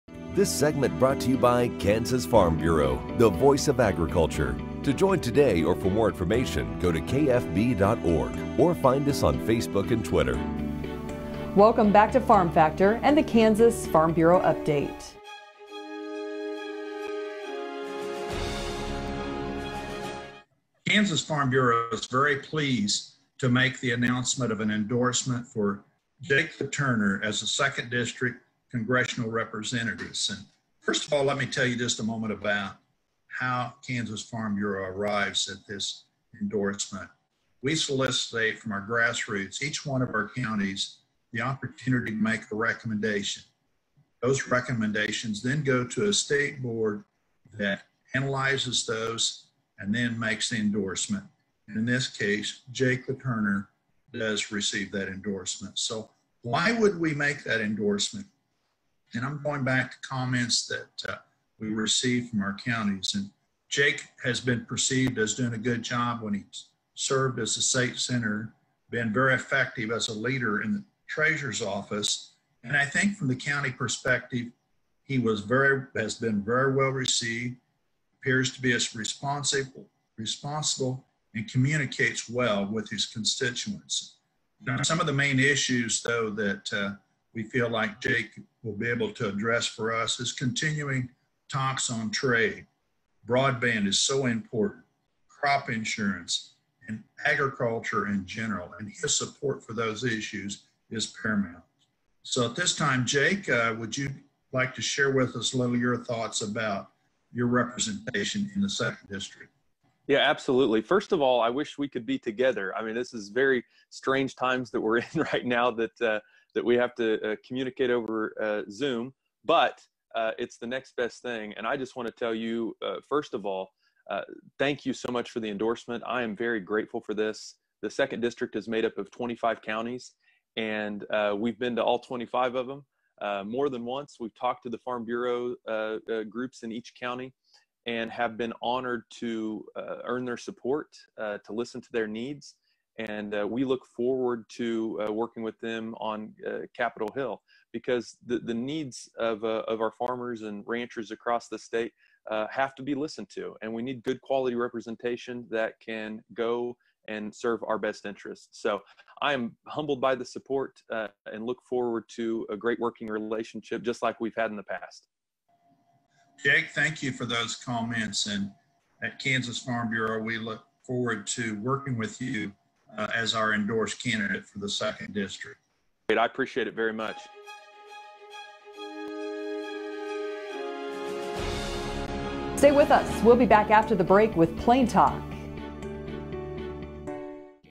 Farm Factor 06162020 KFB Update
[music]
Richard Felts: Kansas Farm Bureau is very pleased to make the announcement of an endorsement for Jake LaTurner as the Second District Congressional Representative.
First of all, let me tell you just a moment about how Kansas Farm Bureau arrives at this endorsement. We solicit from our grassroots, each one of our counties, the opportunity to make the recommendation. Those recommendations then go to a state board that analyzes those and then makes the endorsement. In this case, Jake LaTurner does receive that endorsement. Why would we make that endorsement?
I'm going back to comments that we received from our counties. Jake has been perceived as doing a good job when he served as a state senator, been very effective as a leader in the treasurer's office, and I think, from the county perspective, he has been very well received. Appears to be responsible and communicates well with his constituents.
Now, some of the main issues, though, that we feel Jake will be able to address for us is continuing talks on trade. Broadband is so important, crop insurance, and agriculture in general. His support for those issues is paramount. At this time, Jake, would you like to share with us a little of your thoughts about your representation in the second district?
Jake LaTurner: Yes, absolutely. First of all, I wish we could be together. I mean, this is very strange times that we're in right now that we have to communicate over Zoom, but it's the next best thing. I just want to tell you, first of all, thank you so much for the endorsement. I am very grateful for this. The second district is made up of 25 counties. We've been to all 25 of them more than once.
We've talked to the Farm Bureau groups in each county and have been honored to earn their support, to listen to their needs. We look forward to working with them on Capitol Hill because the needs of our farmers and ranchers across the state have to be listened to. We need good quality representation that can go and serve our best interests. I am humbled by the support and look forward to a great working relationship, just like we've had in the past.
Richard: Jake, thank you for those comments. At Kansas Farm Bureau, we look forward to working with you as our endorsed candidate for the second district.
Jake LaTurner: I appreciate it very much.
[music]
[00:02:50] [END OF AUDIO]